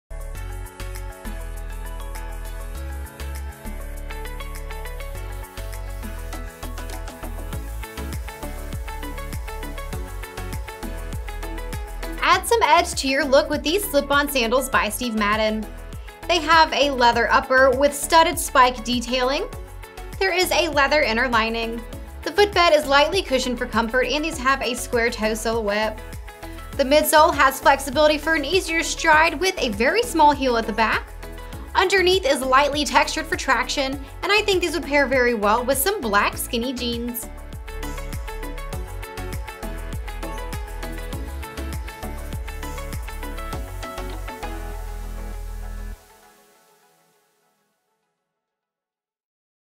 Steve Madden Laverne Sandal SKU: 9561268
The Steve Madden&reg; Laverne Sandal are a pair of sandals that will go with anything from denim to your favorite dress.
Open toe and back.
Vamp strap for support and comfort.
Padded footbed for all day wear.
Low heel.
Leather upper, lining, and footbed.
Synthetic outsole.
Imported.